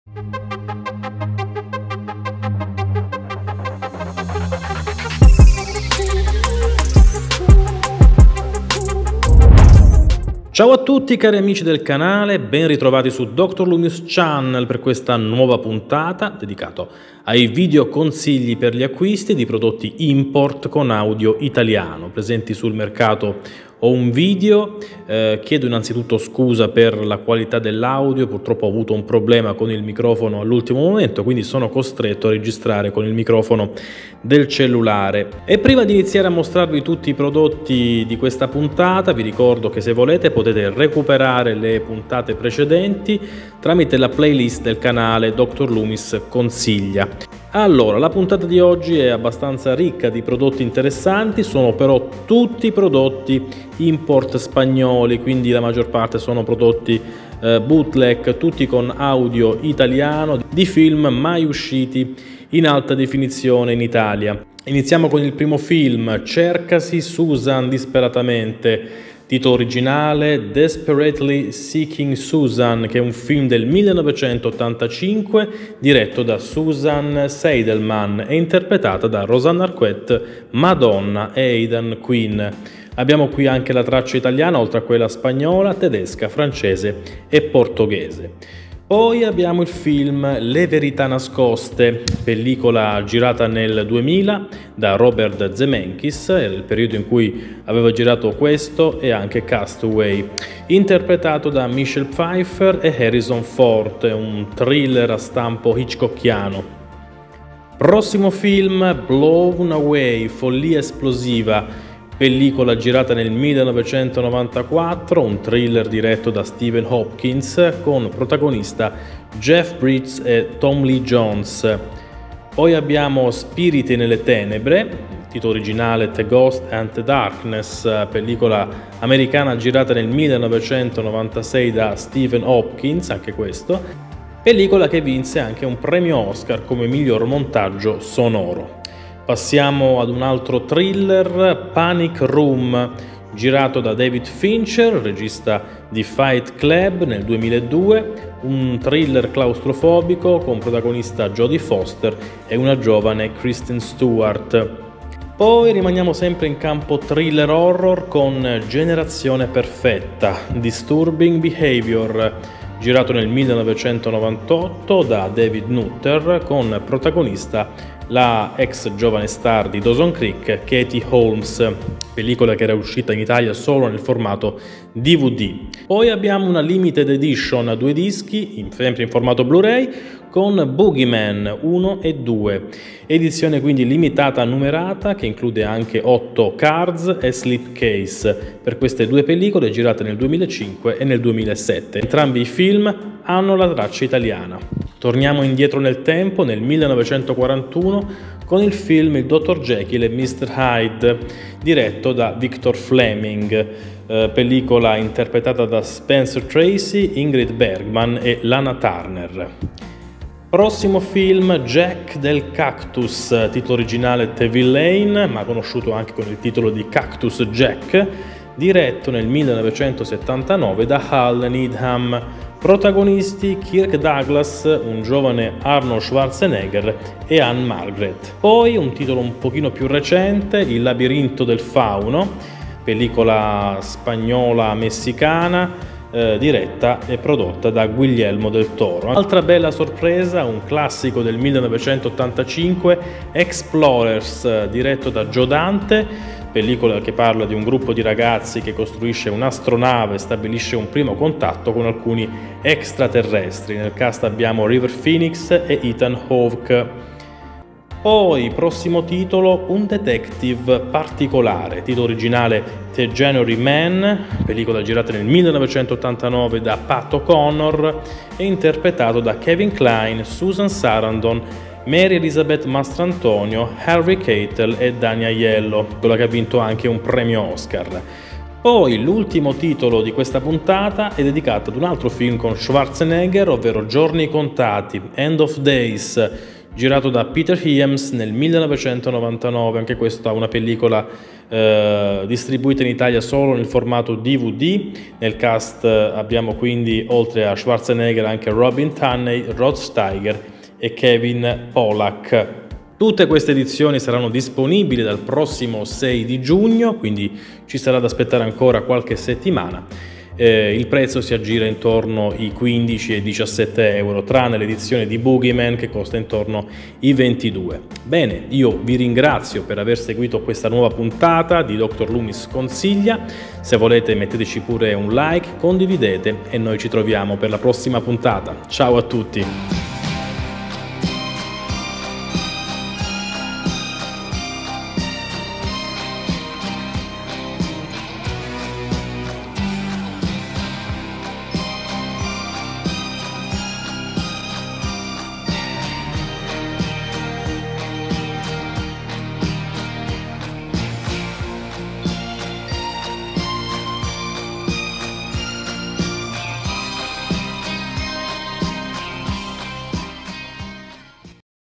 DR.LOOMIS CONSIGLIA #09: Edizioni Import Bluray con Audio Italiano🇮🇹 [Maggio '23]
Nuovi consigli Import del mese di Maggio

Nota: segnalo che il film con Madonna è Who's That Girl (erroneamente l'ho confuso con "Crcasi Susan Disperatamente")

Unitevi alla Chat Telegram per scoprire le migliori promo Home Video